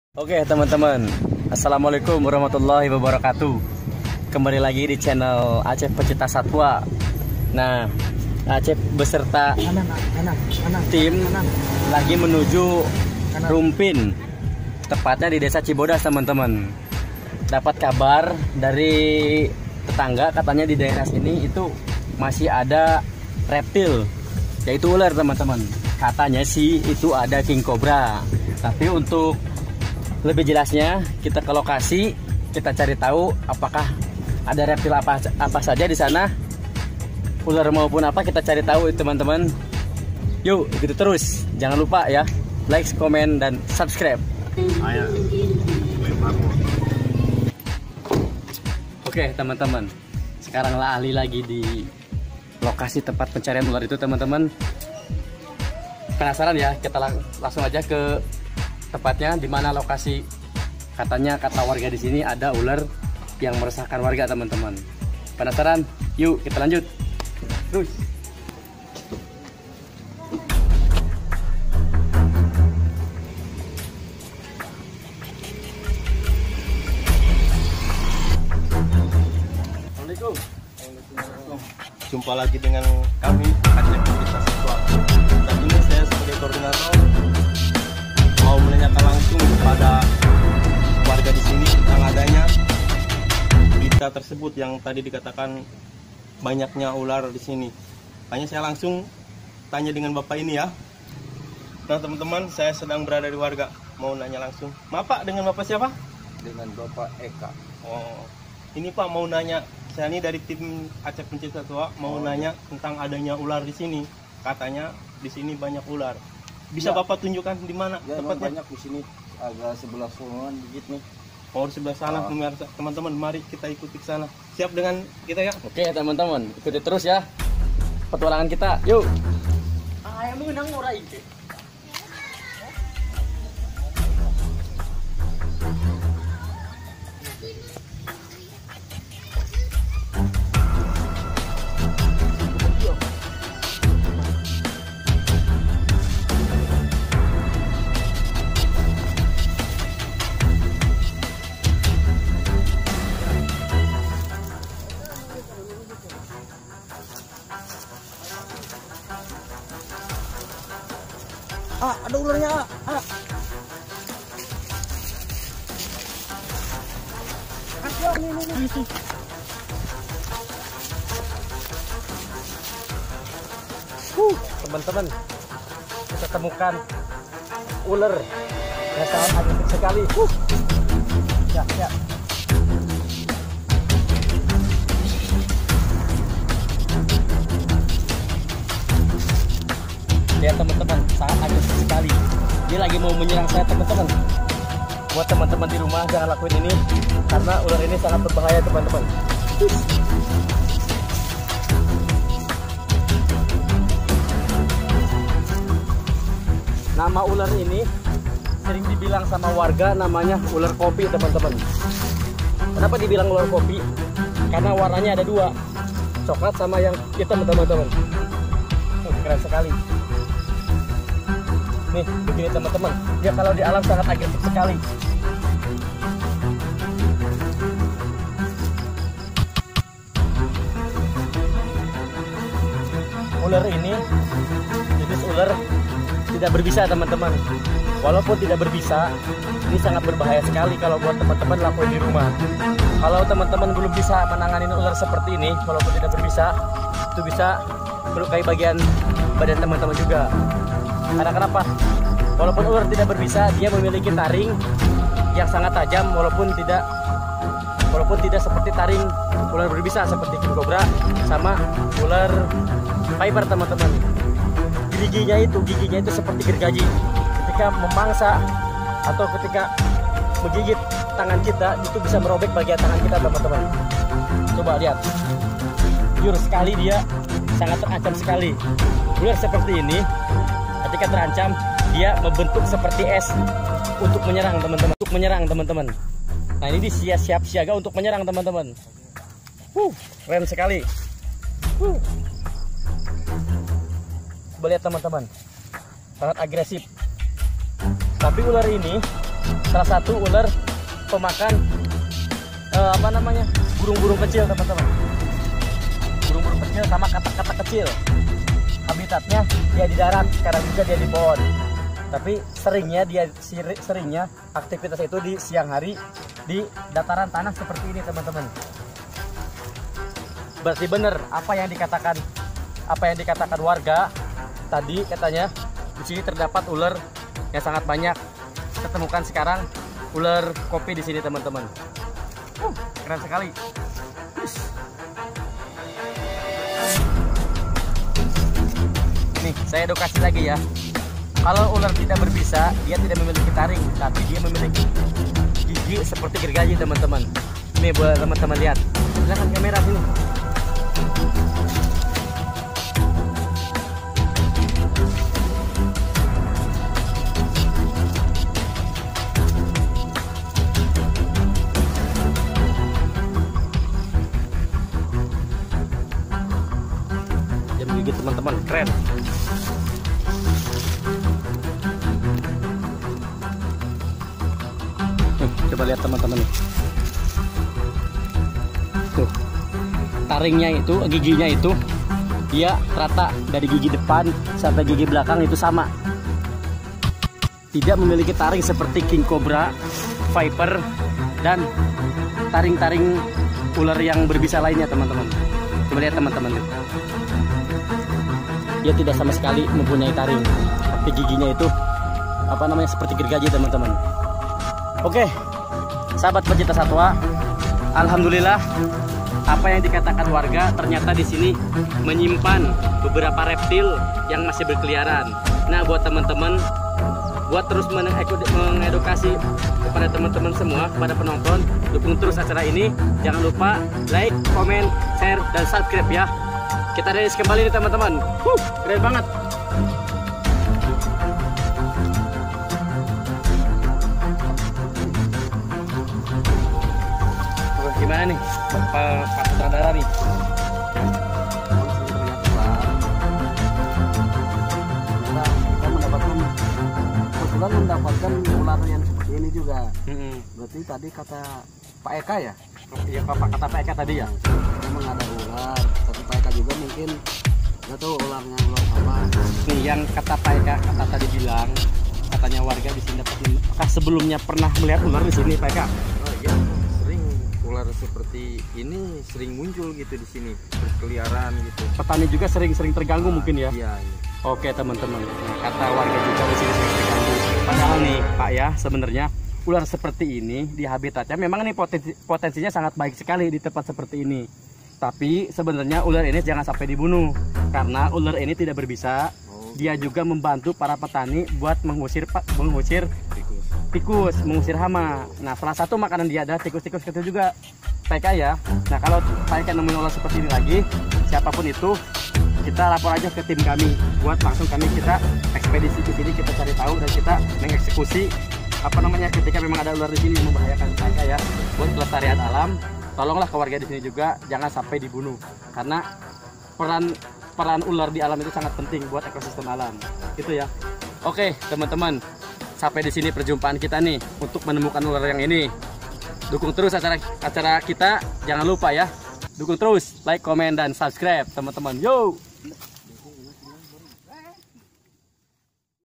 Menemukan hewan si ular gesit di perkebunan warga
Di lokasi desa ci bodas terdapat beberapa hewan peliharaan dan reptil tapi yang kami temukan si gesit ular copi peng huni hutan dan perkebunan ci bodas kecamatan rumpin